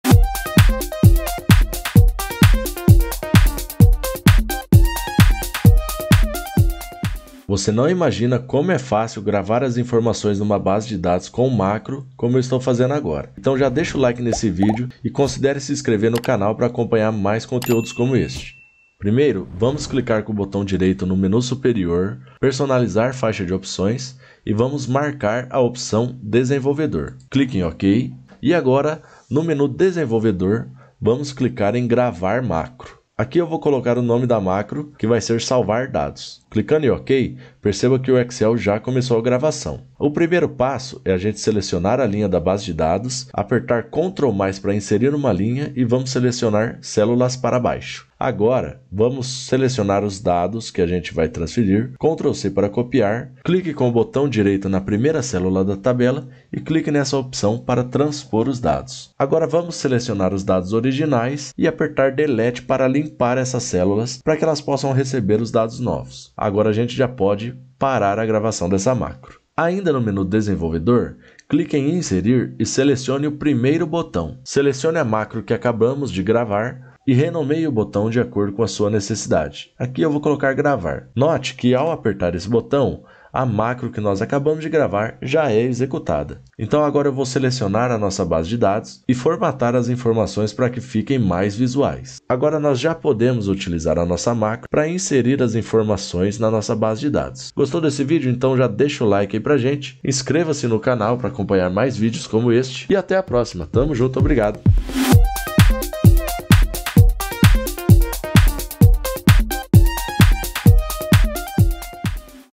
MACRO VBA para gravar informações de um formulário em uma base de dados no Excel
Olá, neste vídeo eu explico muito rapidamente como você pode habilitar a aba desenvolvedor para gravar macros no Excel. A explicação se baseia em uma macro simples e fácil que copia informações de uma local, como um formulário por exemplo e cola em outra planilha, como uma base de dados.

Isso pode ser utilizado para automatizar rotinas de cadastro e garantir a confiança das informações.

Se gostou e quer mais vídeos como este, comente aqui embaixo pra eu saber que esse conteúdo é relevante.